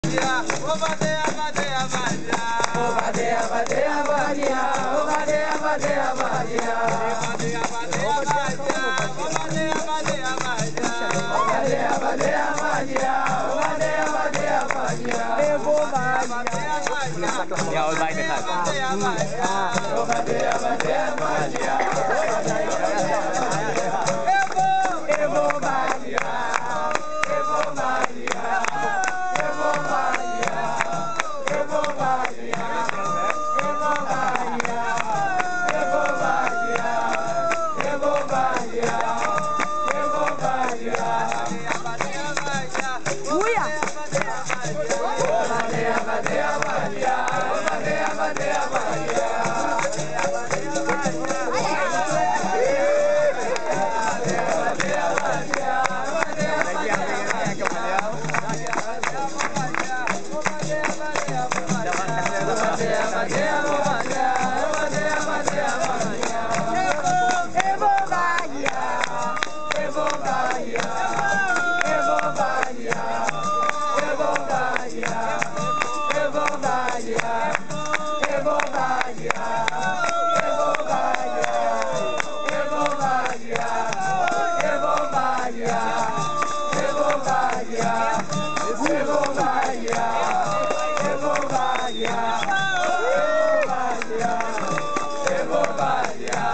jogo de c.m roco & c.m pinoquio cdo israel
Roda de rua
mestre edan cdo israel
6.2.15